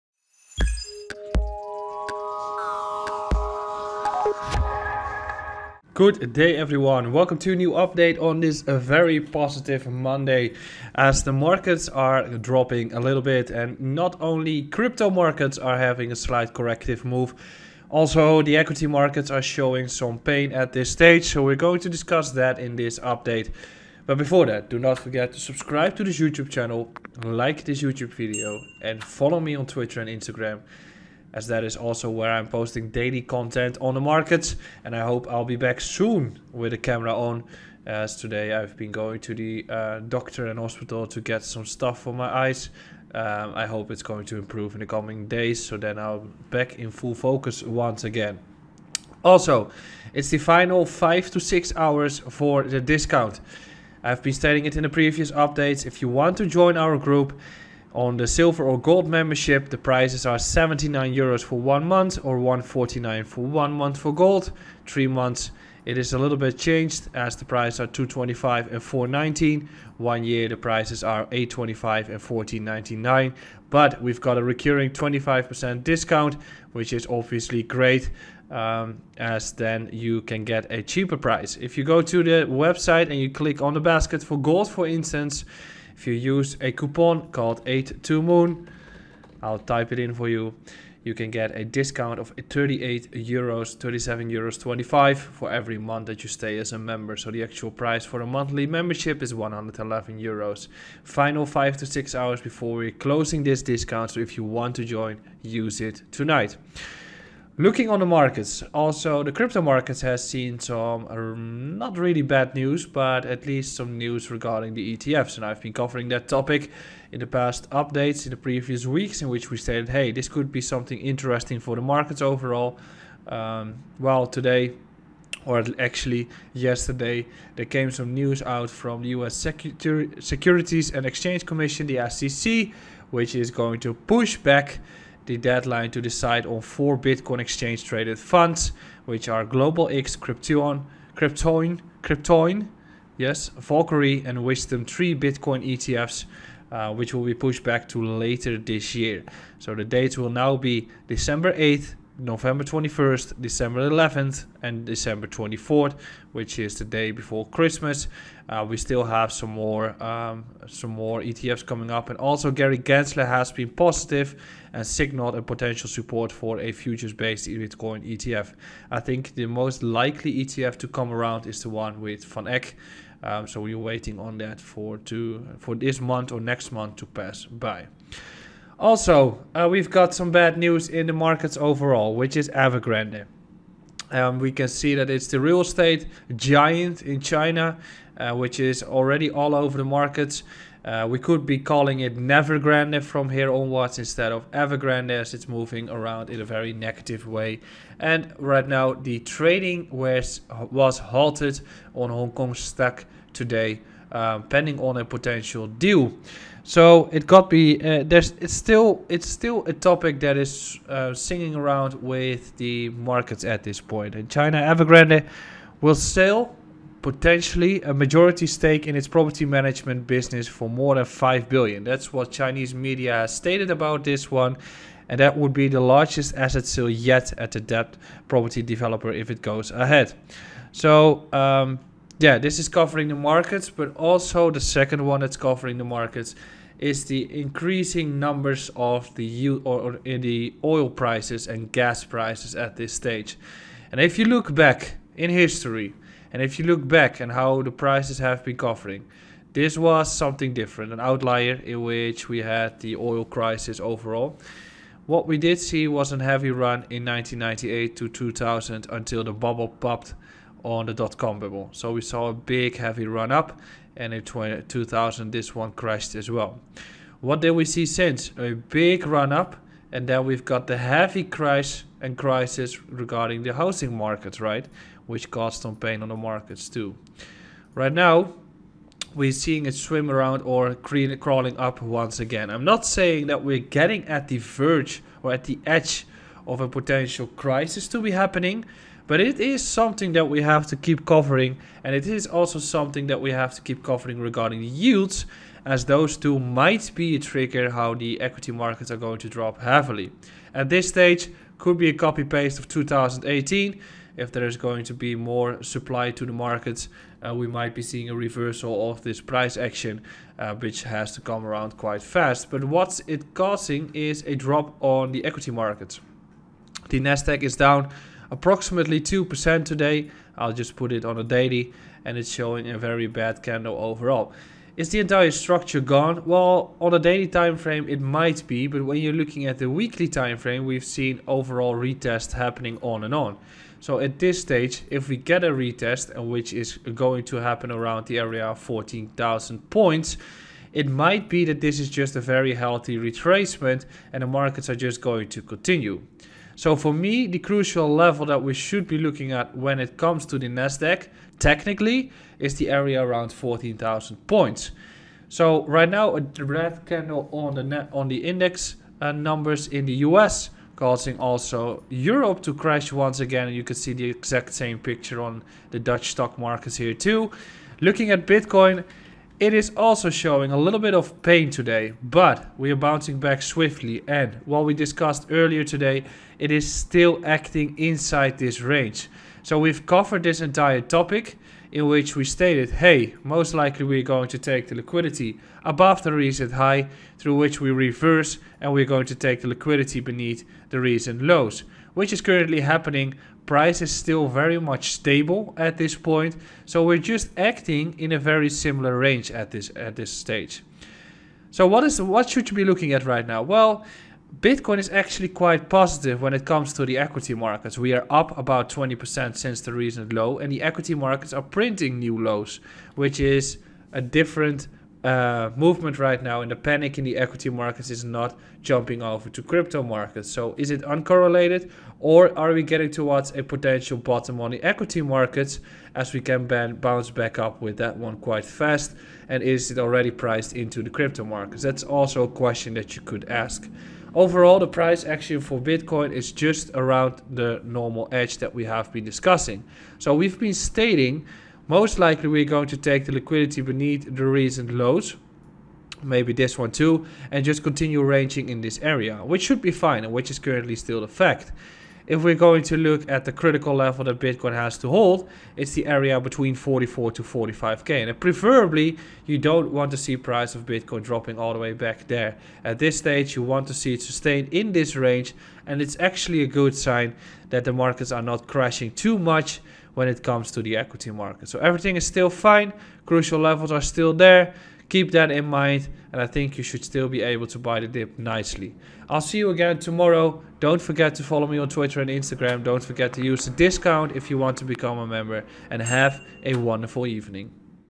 (N)EVERGRANDE CRISIS: BEGINNING OF THE END? 📉 WHAT ABOUT BITCOIN?!
More news from Evergrande came out, together with some updates from potential future Bitcoin ETFs. Is there a crisis glooming over the markets? How much time do we have left and what about Bitcoin? Let's take a look at some updates and Bitcoin price action.

Where do I buy my Bitcoin and crypto? My primary exchange is Bitvavo.

0:00 Intro
0:49 Final day of discount weekend
1:47 Recent news
6:09 Bitcoin
7:58 Outro